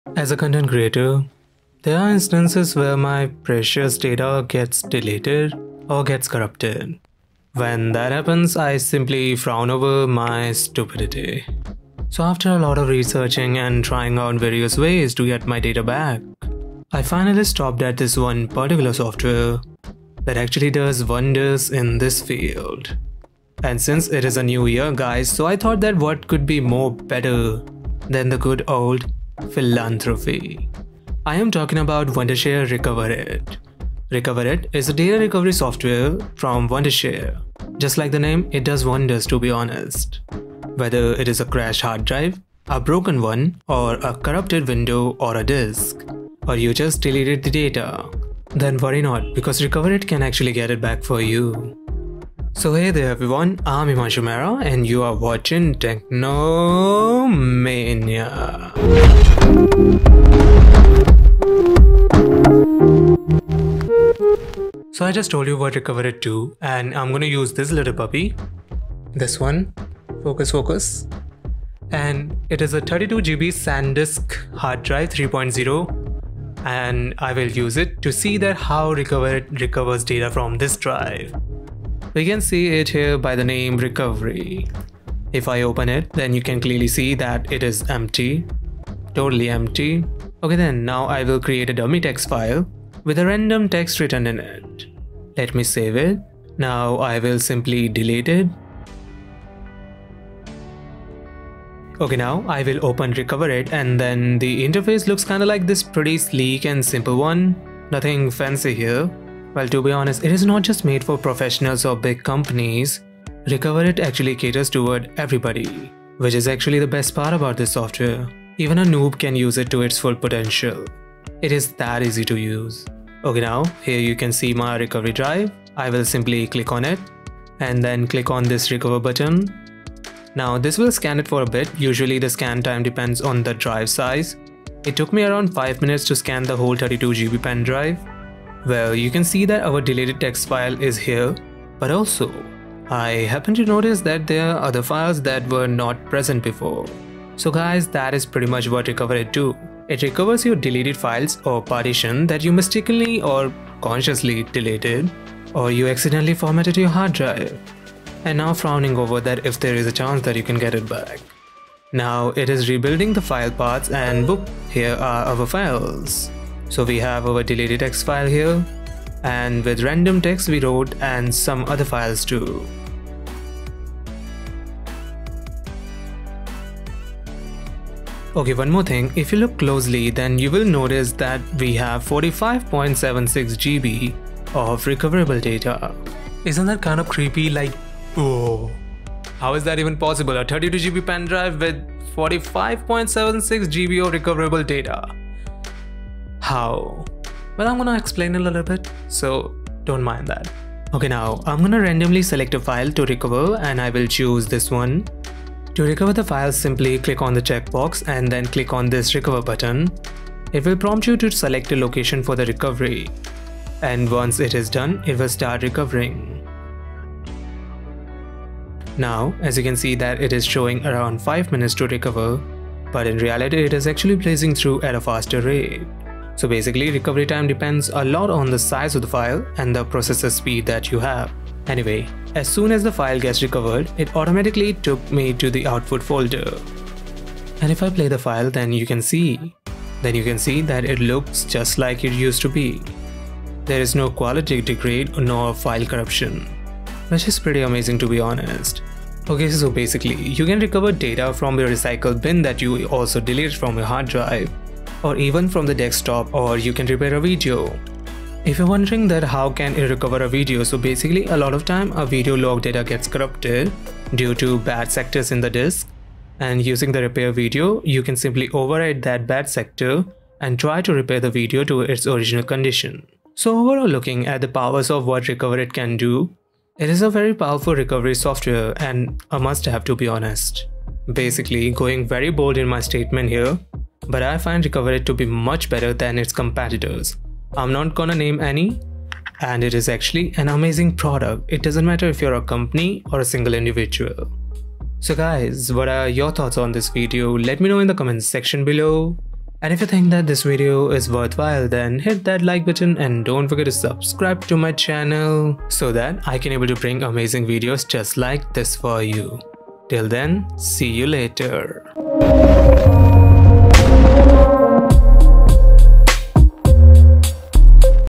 Wondershare Recoverit ( The Only Data Recovery Software You Need )
What is better to start the year rather than the good old philanthropy.
Today I will show you the god tier way to save your data that you adore.
Using The God Tier Recovery App from Wondershare.
The Maker of the best and Easiest Video Editing software Filmora and Filmora 9